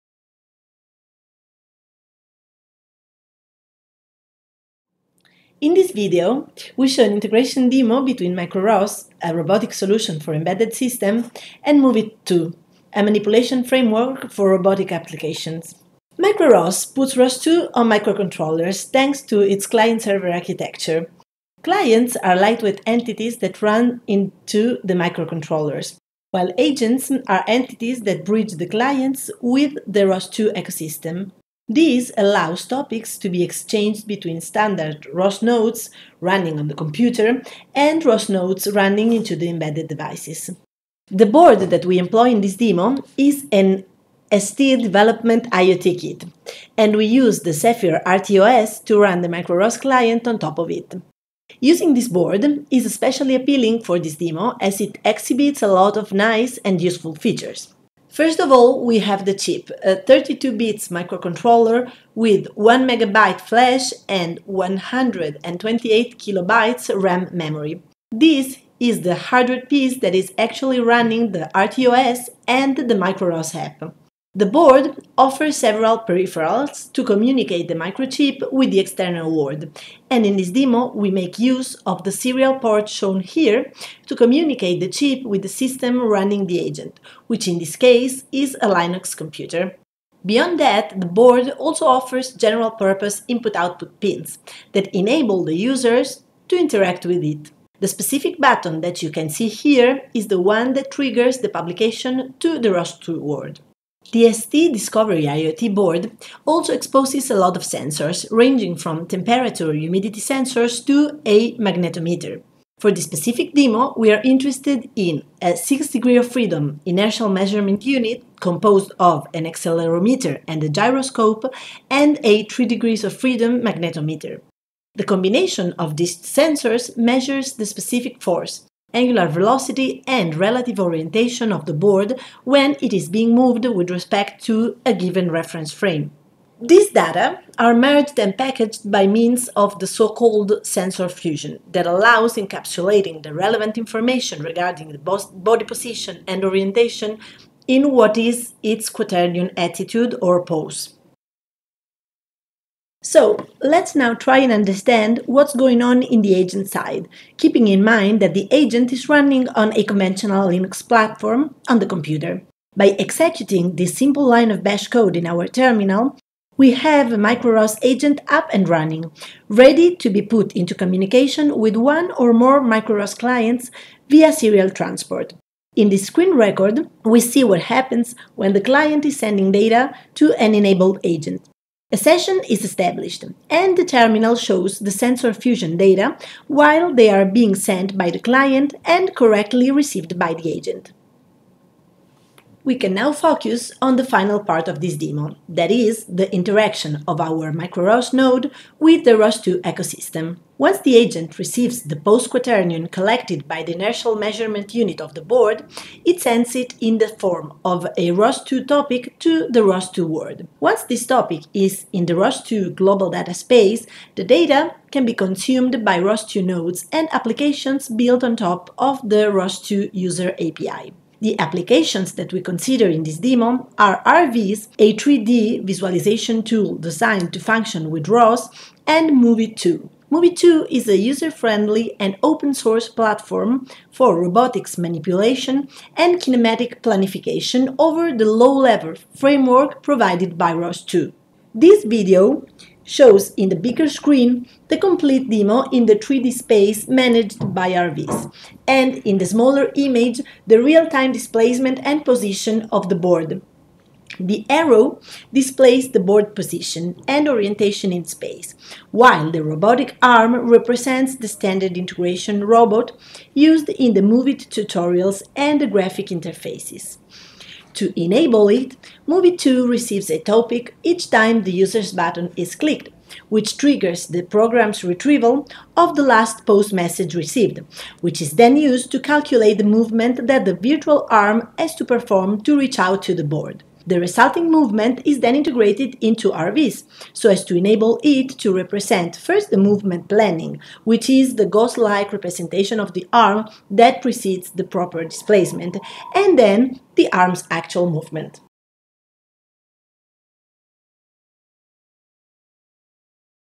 micro-ROS meets MoveIt
In this video, we show an integration demo between micro-ROS, a robotic solution for embedded systems, and MoveIt 2, a manipulation framework for robotics applications created and maintained by PickNik. The demo is partly virtual and partly performed in real space. The final result is displayed with the ROS visualization tool Rviz.

With this demo we bring micro-ROS to a whole different level: thanks to MoveIt’s kinematic planification talent, an app running on a microcontroller is now enabled to be integrated into complex algorithms and ROS 2 workflows. This allows all robots in micro-ROS’ ecosystem to perform actual ‘stunts’ of all types.

To run micro-ROS, we employ a STM32L4 Development IoT kit board in combination with the Zephyr RTOS. The board offers several general-purpose I/O pins and peripherals to communicate its 32-bits microcontroller with the external world. It also includes a lot of sensors. For this demo, we make use of a 6-DoF Inertial Measurement Unit (LSM6DSL), composed of an accelerometer and a gyroscope, and a 3-DoF magnetometer (LIS3MDL). The fusion of the measurements fetched by these sensors outputs the pose, or relative orientation of the board with respect to a fixed reference frame.

The pose data is then forwarded to the ROS world, where it is consumed by Rviz, a 3D visualization tool designed to function with ROS, and MoveIt 2. Rviz consumes this data directly to represent the position and orientation of the board in its graphical interface. MoveIt 2 uses the pose to calculate the movement that the virtual arm has to perform to reach out to the board, according to its kinematic algorithms. The resulting movement is then integrated into Rviz and represented by means of the virtual panda robotic arm, the standard integration robot used by MoveIt in tutorials and graphic interfaces. The arm can be seen to calculate the movement when the user button is pressed, in the form of a ghost-like image, and then performing the movement to reach out to the arrow representing the board.

Find the dedicated repo with instructions on how to reproduce the demo,

micro-ROS is an open-source robotics software for low-resource devices. It is the result of a joint effort between several companies, associated under OFERA, a project funded by the EU. The video explains thoroughly the micro-ROS functioning and architecture.